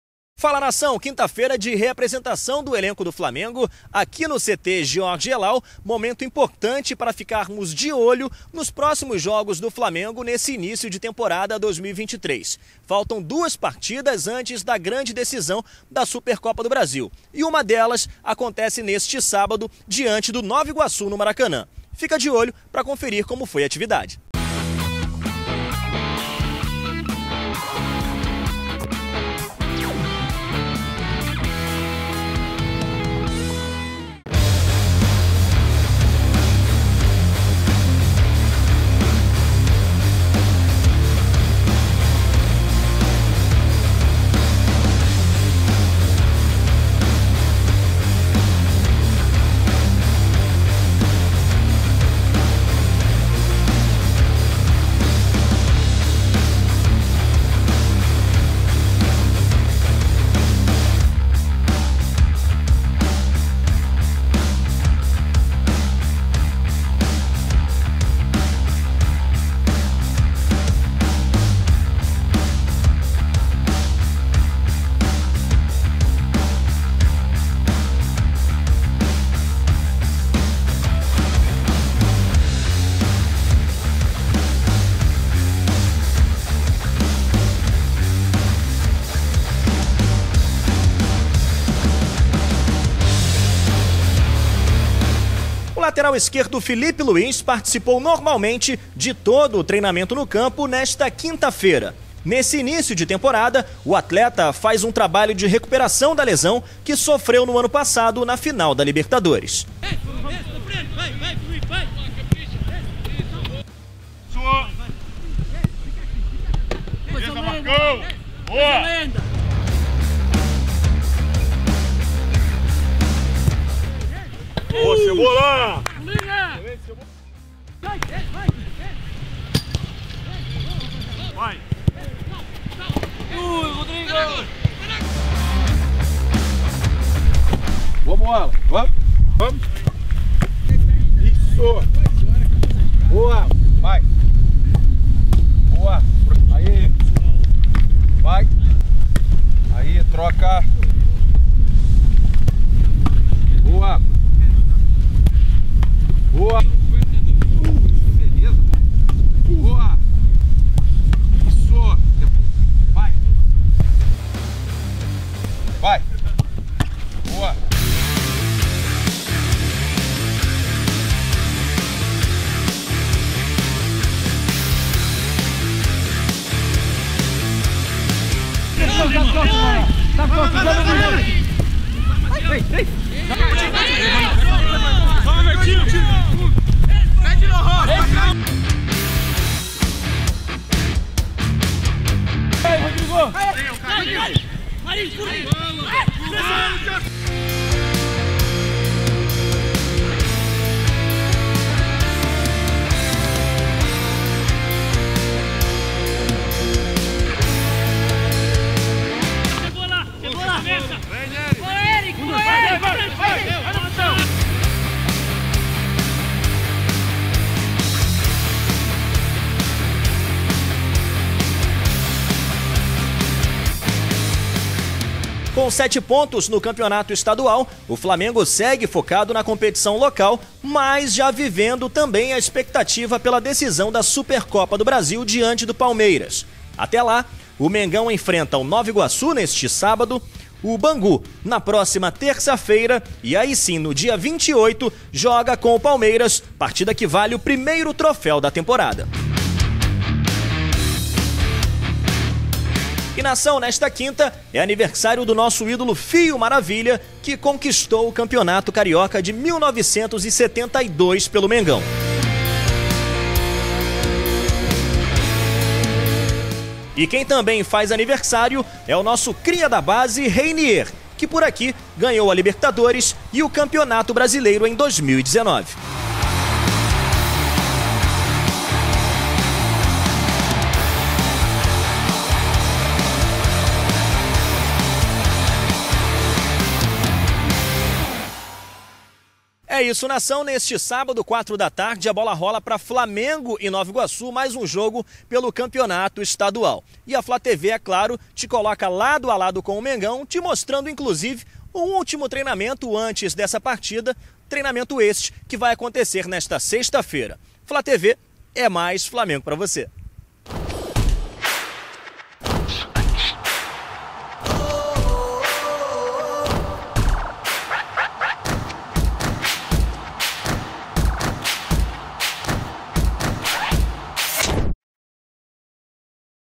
Com Filipe Luís em campo, Flamengo treina de olho no Nova Iguaçu
Fala, Nação!

Nesta quinta-feira (19), o elenco se reapresentou, no Ninho do Urubu, e iniciou a preparação para o confronto com o Nova Iguaçu, no Maracanã, pela terceira rodada da Taça Guanabara. A novidade de hoje foi a participação do lateral esquerdo Filipe Luís no treinamento em campo. Neste início de temporada, o atleta faz um trabalho de recuperação da lesão que sofreu na libertadores. Confira!